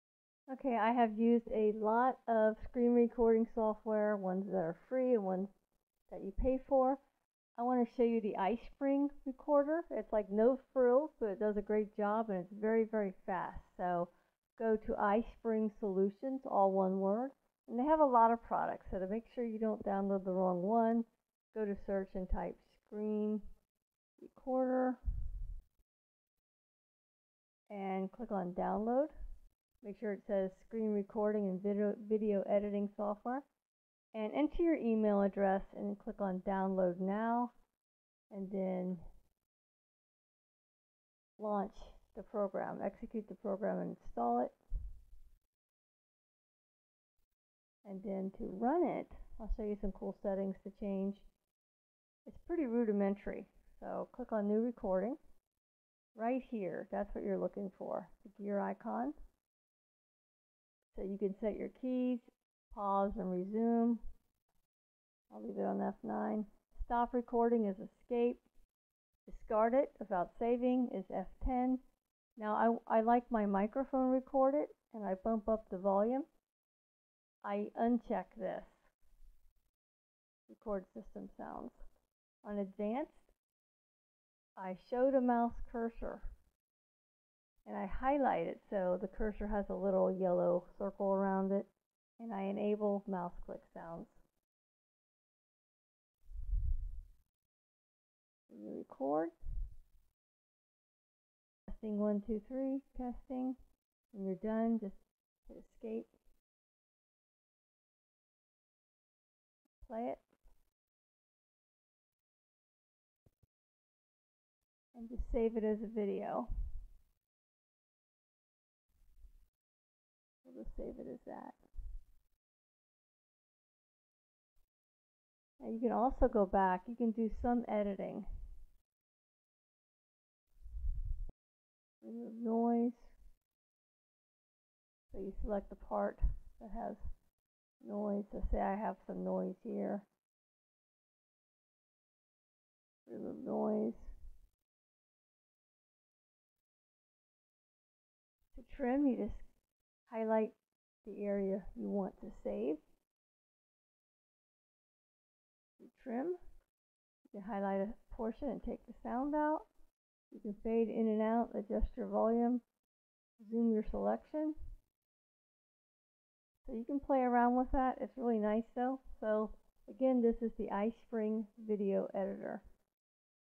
iSpring Screen Recorder and Video Editor - FREE, Fast and Easy
Super easy, no-hassle screen recorder. Creates wmv files.  Records your screen and audio.